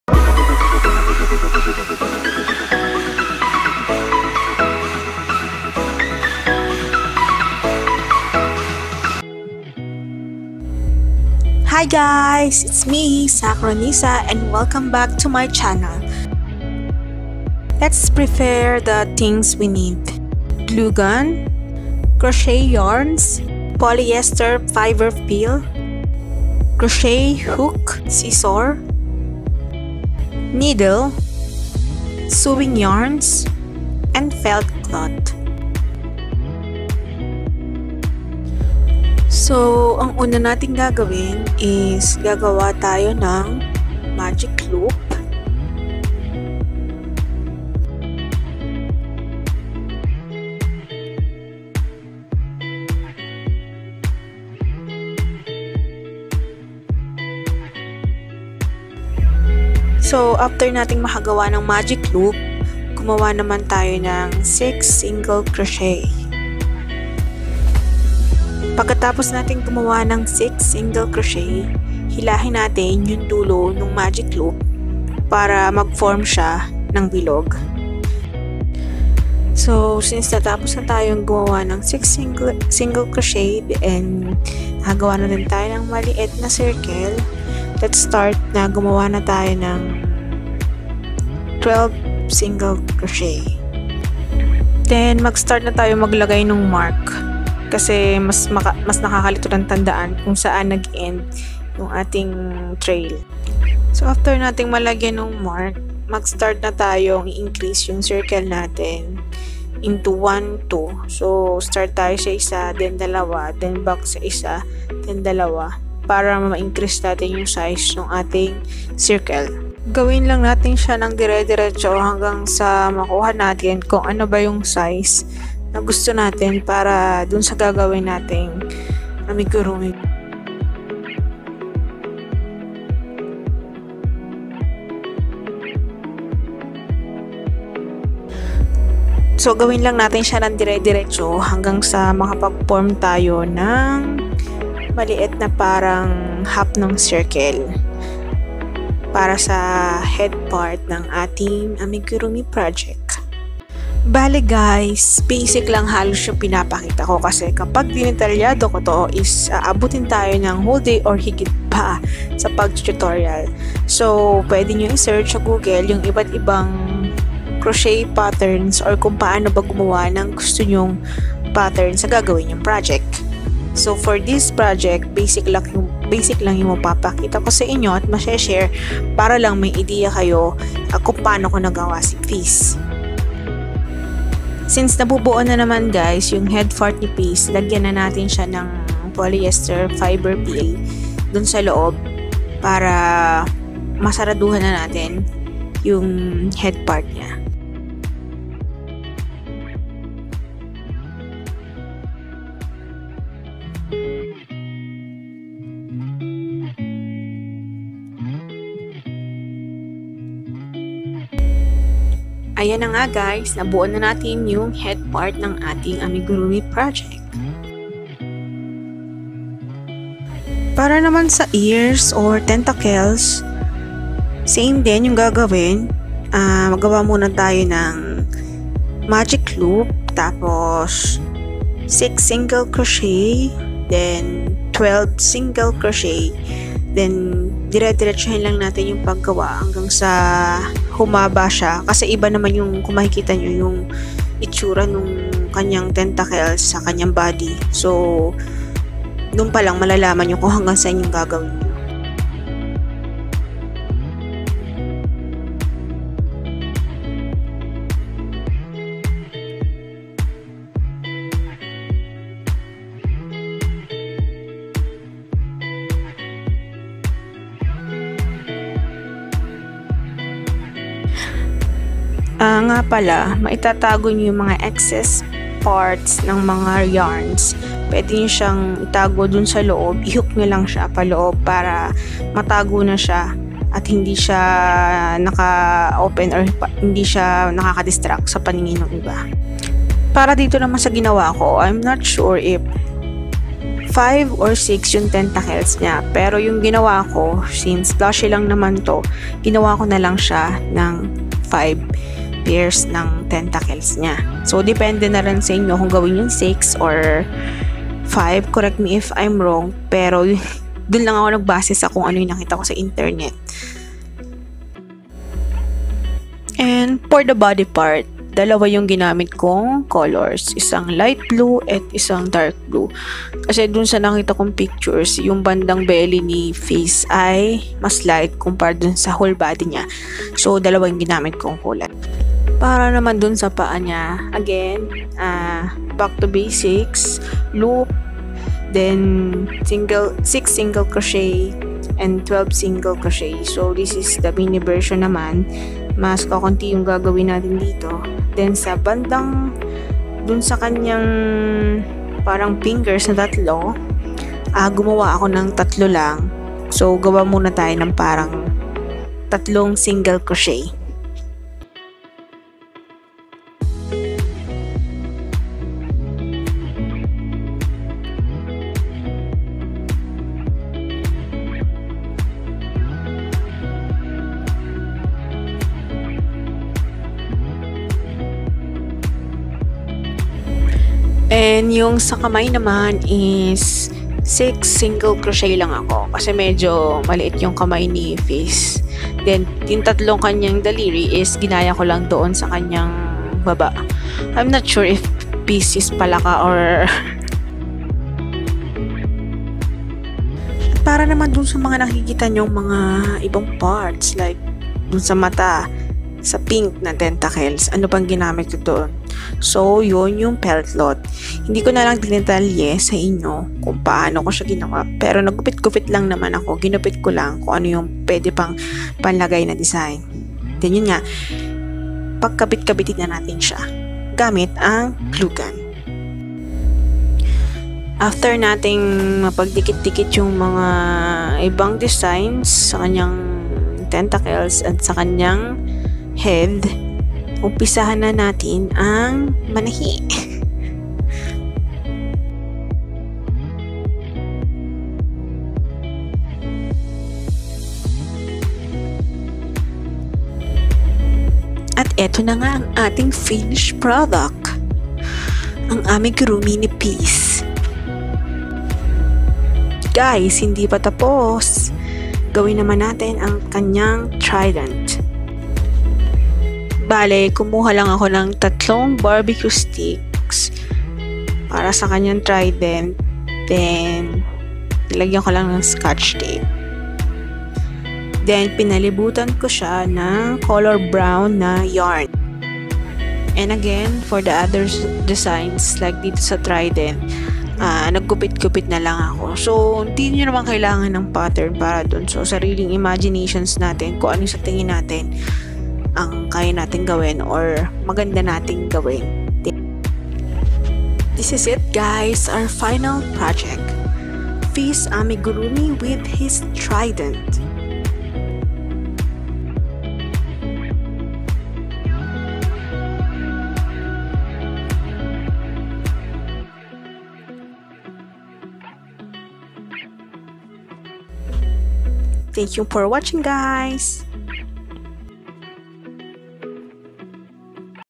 [VLOG#4] AMIGURUMI - FIZZ (LEAGUE OF LEGENDS)
Hi guys! Let's make an amigurumi of FIZZ one of the legendary hero of LEAGUE OF LEGENDS.